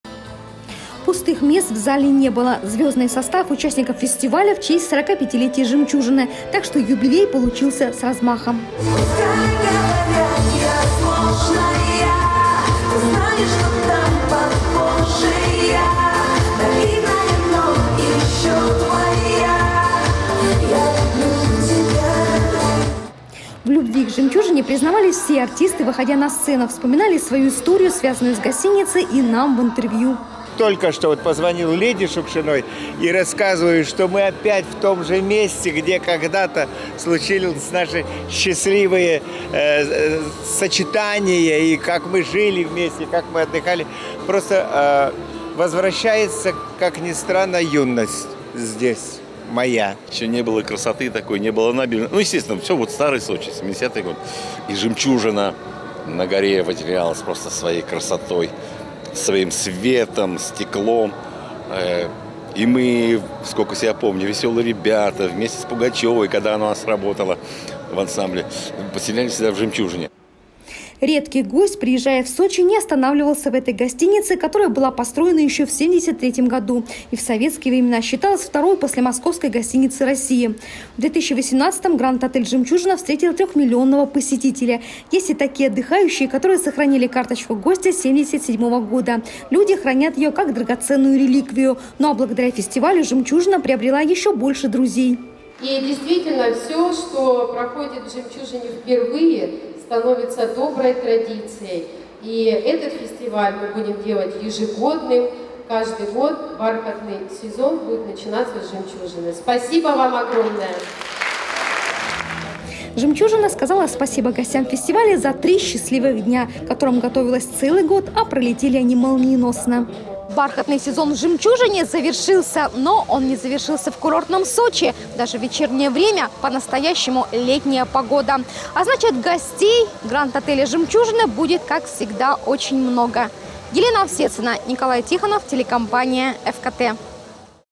Фестиваль"Бархатный сезон" сочинская "Жемчужина" сделает ежегодным. Новости Эфкате Сочи
Александр Буйнов, Катя Лель, Евгений Кемеровский выступили на закрытии фестиваля «Бархатный сезон в Жемчужине». И пообещали поддерживать его ежегодно. Три концертных дня проходили в Летнем театре парка имени Фрунзе.

Пустых мест в зале не было. Звёздный состав участников фестиваля - в честь 45 -летия «Жемчужины». Так что юбилей получился с размахом. В любви к «Жемчужине» признавались все артисты, выходя на сцену. Кто-то вспоминал свою историю, связанную с гостиницей.